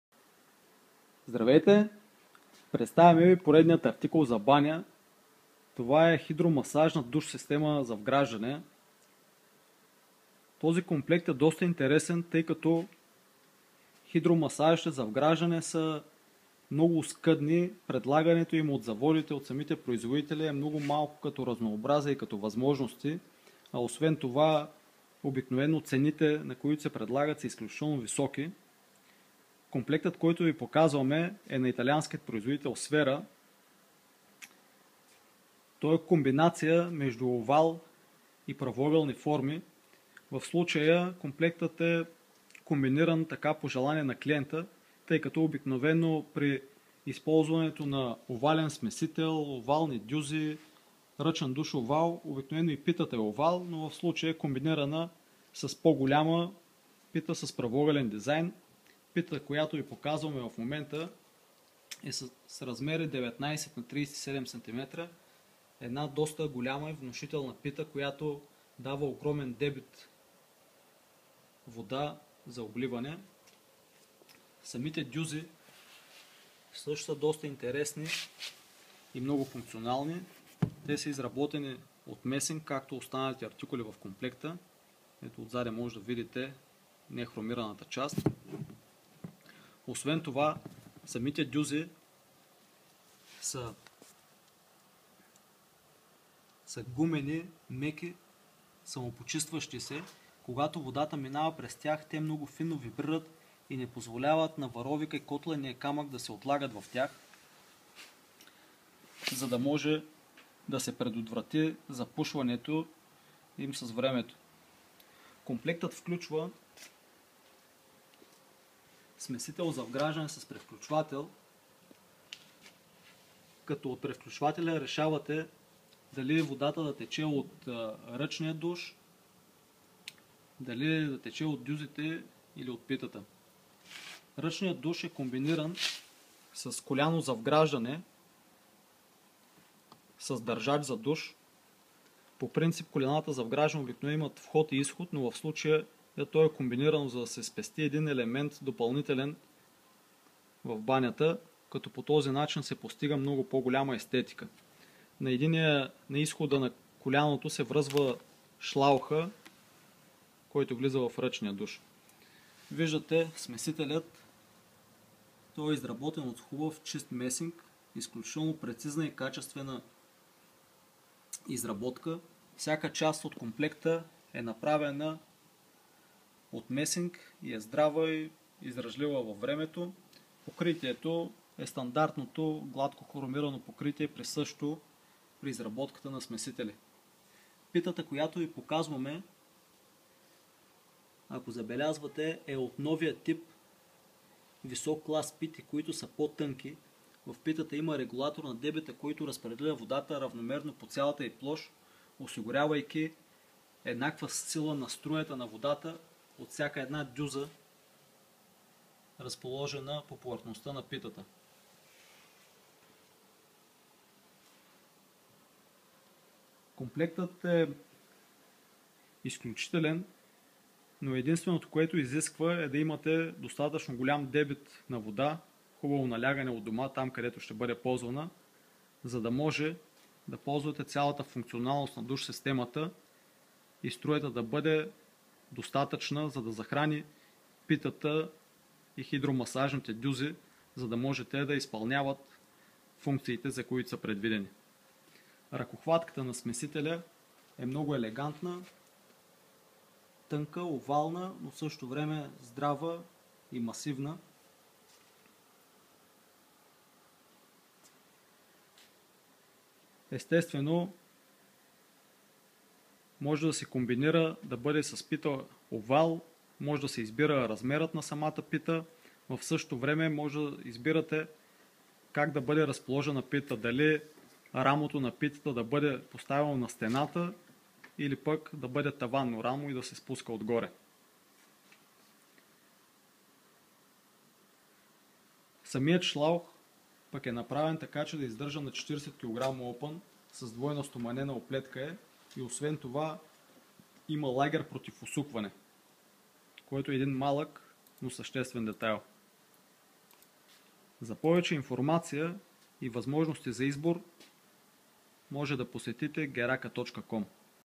Хидромасажна система с дюзи Sphera Tondo
Душ система за вграждане с хидромасажни дюзи за вграждане. Душ системата се състои от смесител за вграждане; рамо за вграждане; душ пита овал 25см; комбинирано коляно с държач за ръчен душ; шлаух; ръчен душ; четири броя хидромасажни дюзи за вграждане.